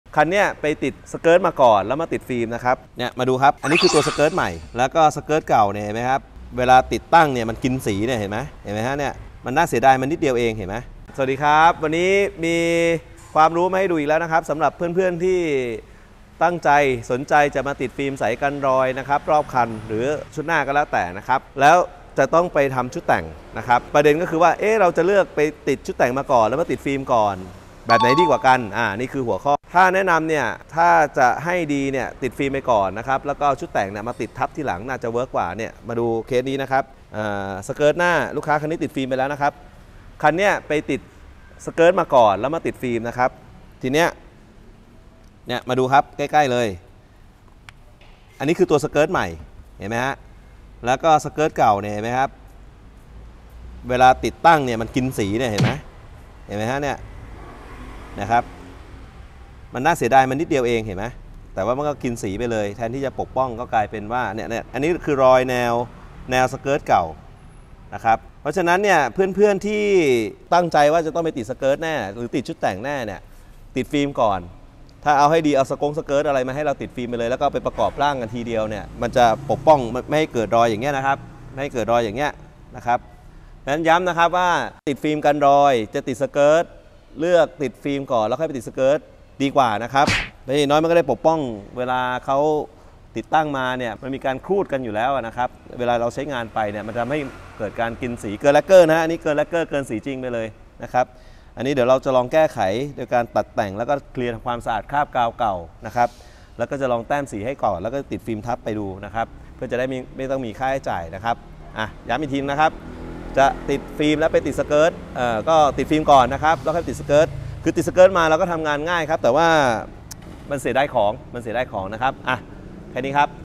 สายแต่งต้องดู ควรติดชุดแต่งก่อนหรือติดฟิล์มกันรอยรถยนต์ก่อน แบบไหนดีกว่ากัน !!
การตกแต่งรถยนต์ด้วยการติดชุดแต่ง หลายคนคิดว่าควรทำให้เรียบร้อยก่อน จึงค่อยทำการปกป้องด้วยการเคลือบเซรามิกหรือติดฟิล์มใสกันรอยรถยนต์เพื่อทำการป้องกันริ้วรอยต่างๆ การเคลือบเชรามิกอาจคิดแบบนั้นและลำดับขั้นตอนการทำแบบนั้นได้ เพราะการเคลือบเซรามิกจะมีคุณสมบัติป้องกันมลภาวะ จึงอาจทำให้การติดตั้งชุดแต่งเป็นไปได้ยาก เพราะบางจุดอาจใช้กาวสองหน้าในการยึดเกาะ ทำให้ติดไม่สนิทหรือติดไม่อยู่หากทำการเคลือบเซรามิกก่อนติดตั้งชุดแต่ง และการติดพาร์ทเสริมนั้น หลายจุดจะติดลงไปบนสีรถเลย หรือมีส่วนที่ชุดแต่งกับสีรถจะสัมผัสกันโดยตรง เมื่อนำรถยนต์ไปใช้งาน ก็จะเกิดการสั่นสะเทือนทำให้ขอบมุมของชุดแต่งเกิดการขูดขีดกับสีรถ จนบางจุดอาจจะเกินชั้นแลคเกอร์เข้าไปถึงสีรถโดยที่ไม่ทันระวัง หรือในบางจุดที่ชุดแต่งมีการปิดทับลงไปทั้งชิ้น จึงมองไม่เห็นถ้าหากเกิดริ้วรอย การติดฟิล์มใสกันรอยจึงเป็นการป้องกันที่ให้ผลดีกว่าการเคลือบเชรามิก

การติดฟิล์มใสกันรอยเป็นการป้องกันริ้วรอยจากชุดแต่ง และที่สำคัญควรติดฟิล์มใสกันรอยรถยนต์ก่อนทำการติดตั้งชุดแต่ง หลายคนเมื่อทำการเปลี่ยนชุดแต่ง จะพบว่าเกิดริ้วรอยบริเวณขอบมุมต่างๆ เมื่อนำชุดแต่งชิ้นใหม่ ที่อาจจะมีการออกแบบที่เล็กลง ไม่สามารถบังริ้วรอยที่เกิดขึ้นได้ จึงควรติดฟิล์มใสกันรอยรถยนต์ก่อนการติดตั้งชุดแต่ง เพื่อไม่ให้สีรถเกิดความเสียหาย

✨Albatros Full Protection Center✨
🚩🚩 รัชดา & นนทบุรี 🚩🚩

📞สนใจสอบถามข้อมูลโทร
📍 Albatros รัชดา

📍 Albatros นนทบุรี